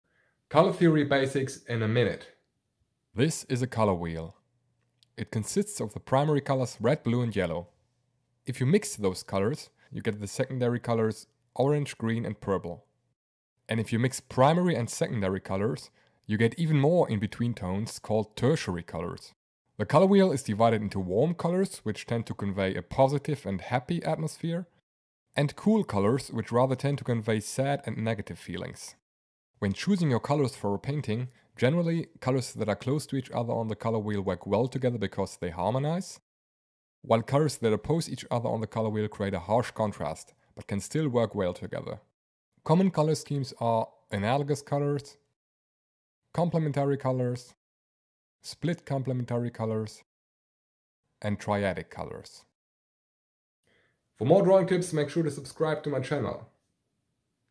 Color Theory in 1 Minute
Color is a complex topic and mastering the use of it takes time and practice. However, understanding the basics of color theory can be quick and easy. The fundamentals of color theory for artists, like the color wheel, primary/secondary colors, warm/cool colors, and color schemes like complementary colors, can be easy to grasp, even for beginners.

Even noobs should be able to improve the color choices of their paintings in 1 minute flat. After watching my video. ;)

MY ART EQUIPMENT:
• Colored Pencils
• Pencil Sharpener
• Brush Pens
• Graphic Tablet
• Video Camera
• Video Lighting